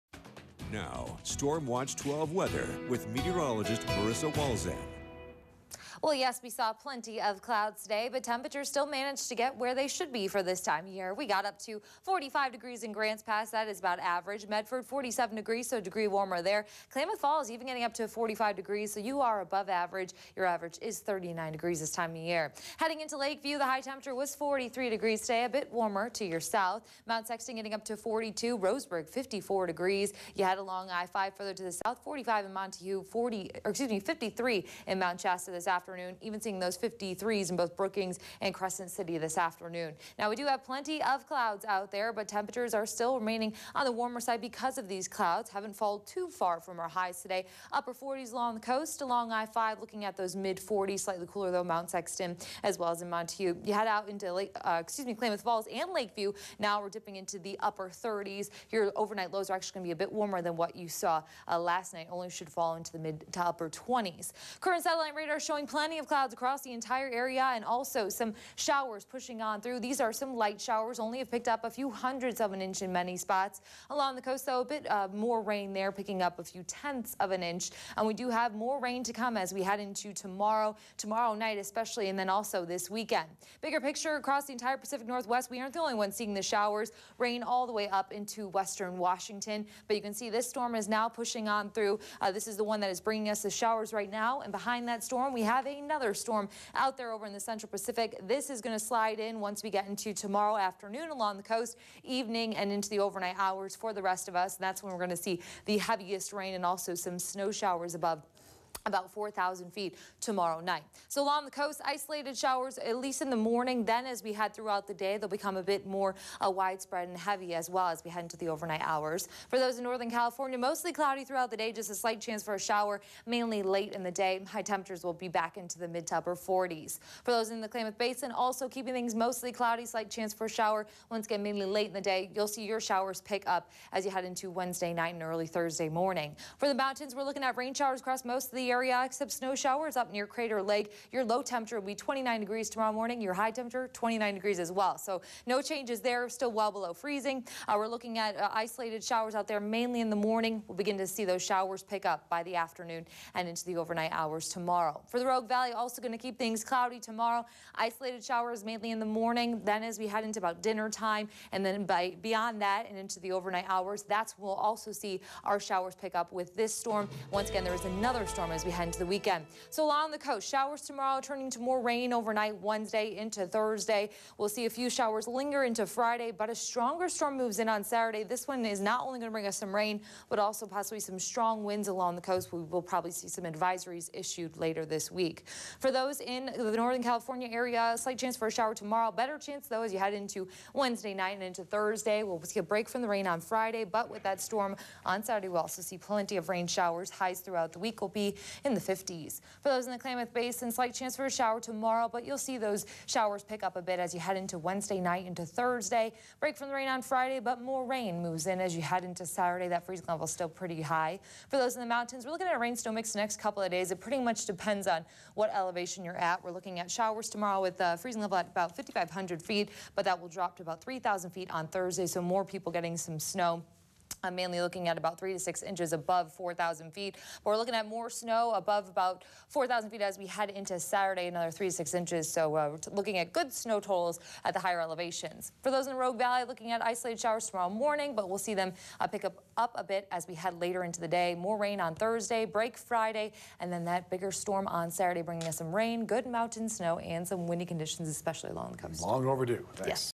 Tuesday Night's Forecast, Jan. 7
Tuesday night's forecast from the StormWatch 12 Weather Center.